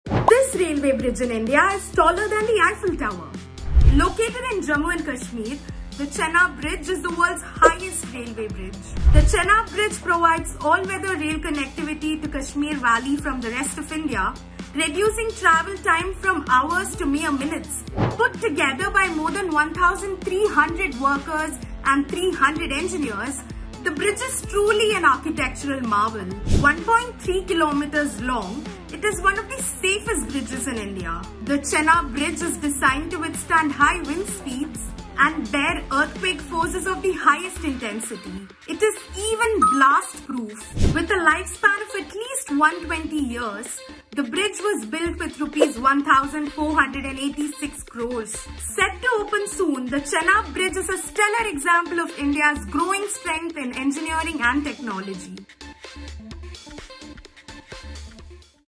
This Indian Bridge is taller than the Eiffel Tower | The Better India
Did you know the chenabbridge is blast-proof and taller than the Eiffel Tower?
Watch the video to learn more about the world's highest railway bridge!